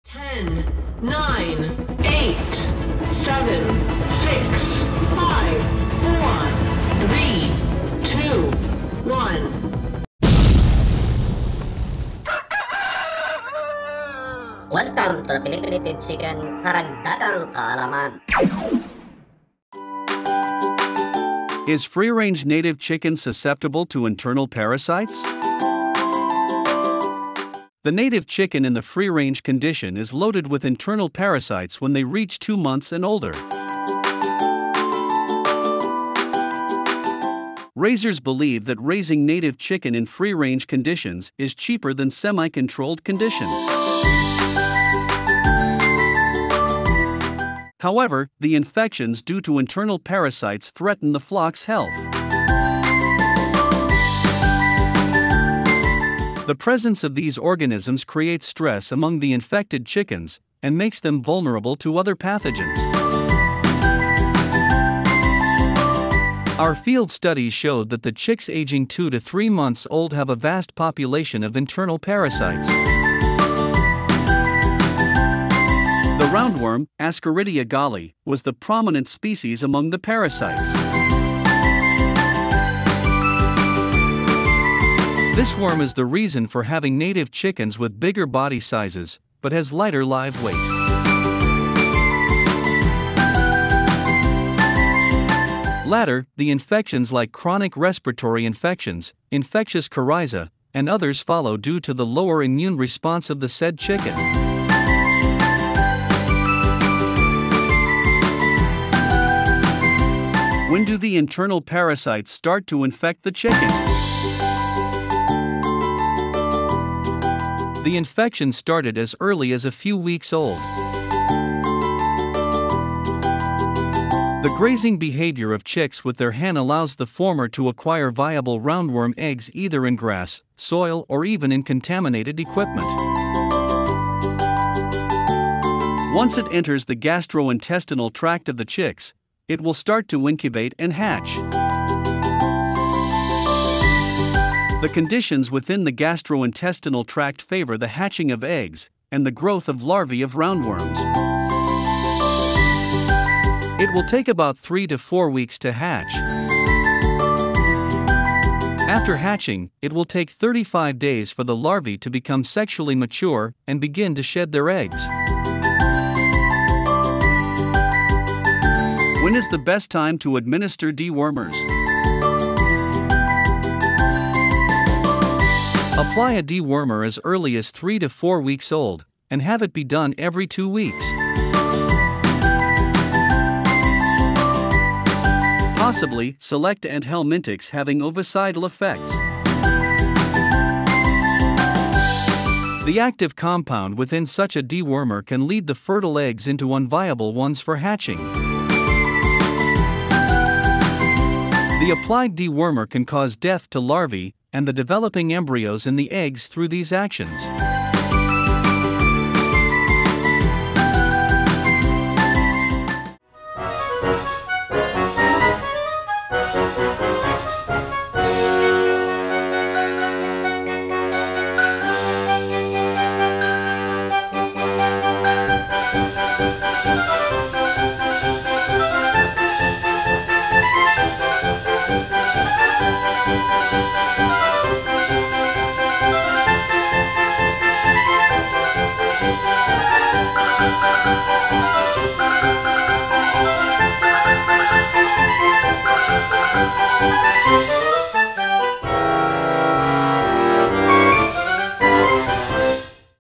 Is free range native chicken, vulnerable to internal parasites?
The videos shown in this channel are supporting the advocacy in the commercial production of common/Philippine native chicken. More information can be found in this